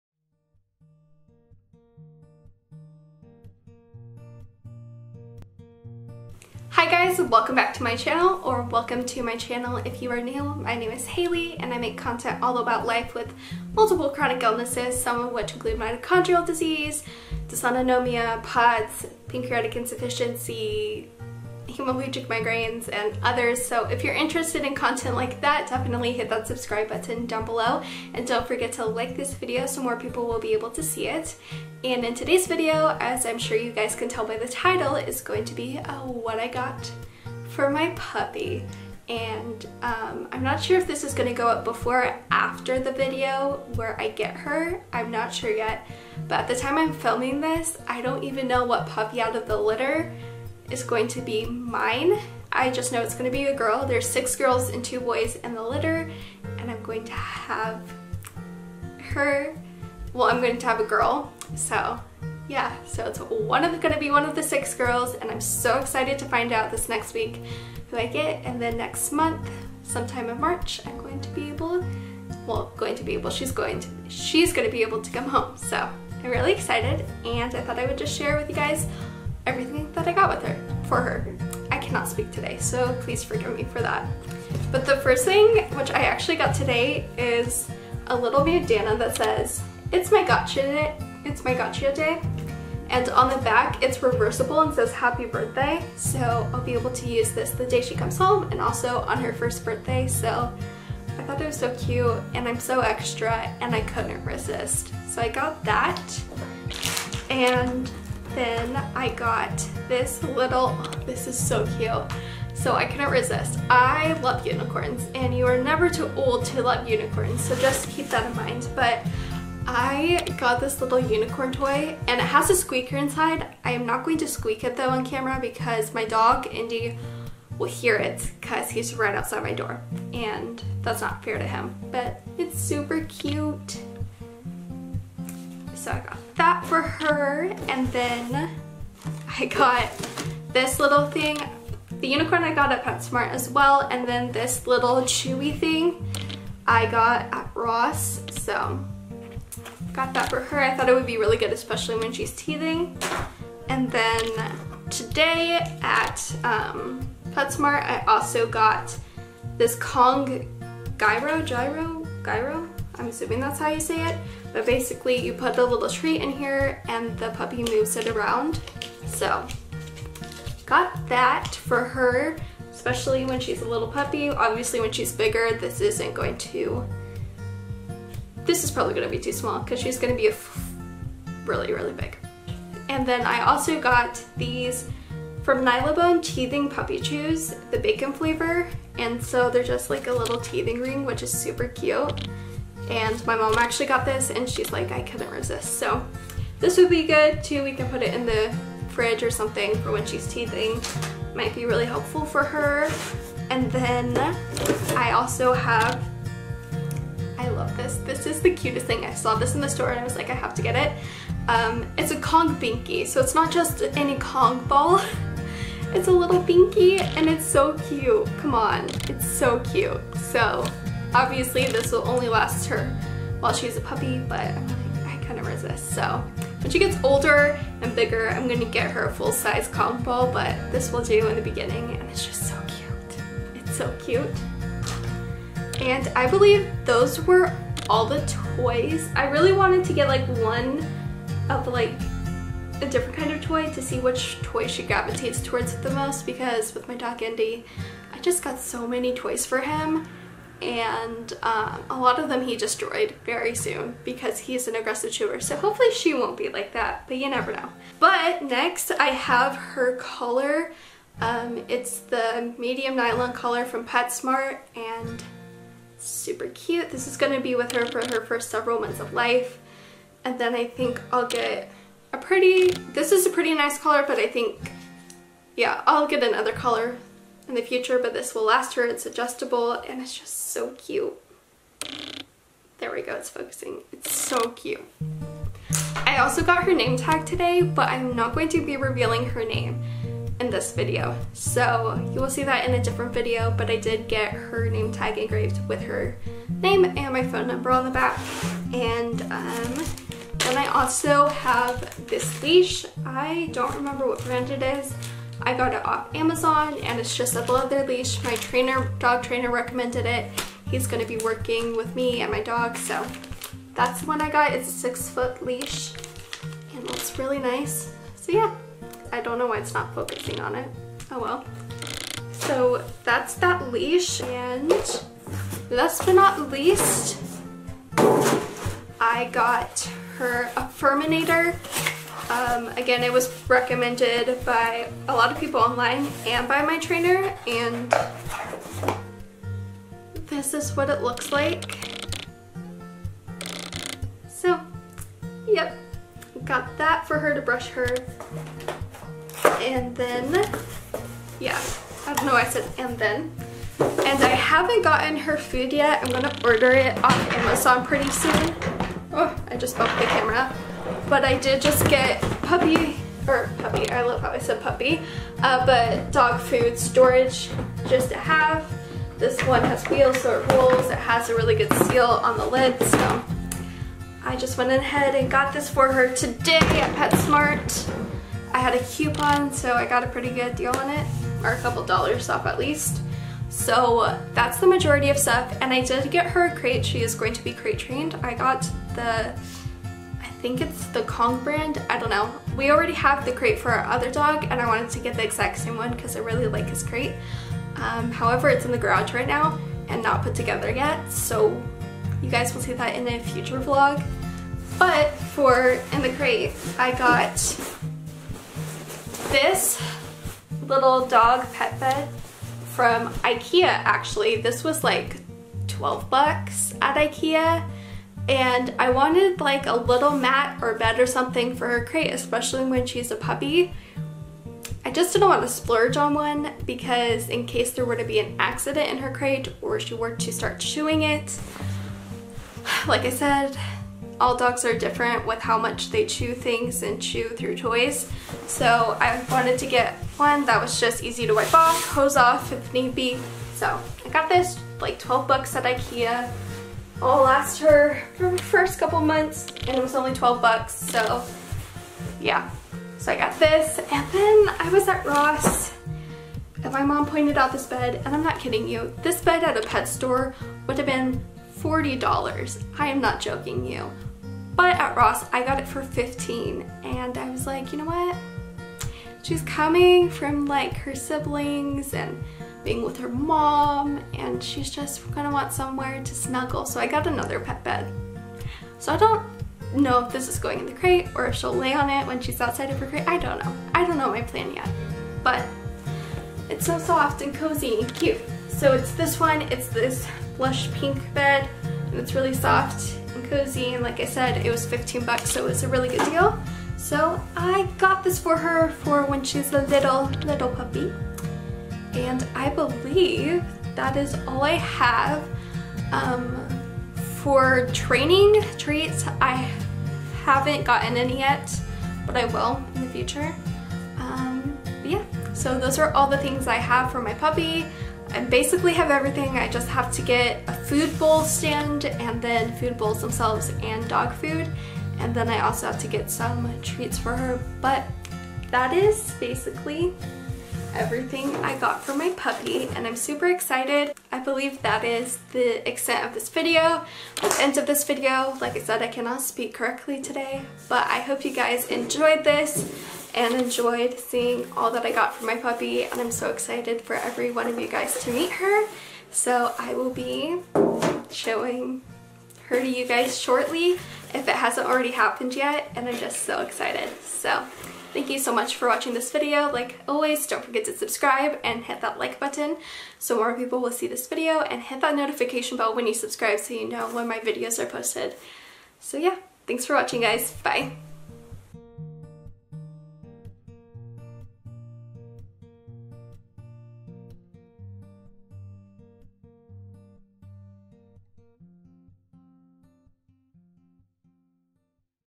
Everything I Got For My New Puppy Haul!
Hey guys! In today's video I share everything I got for my new German shepherd puppy Willow!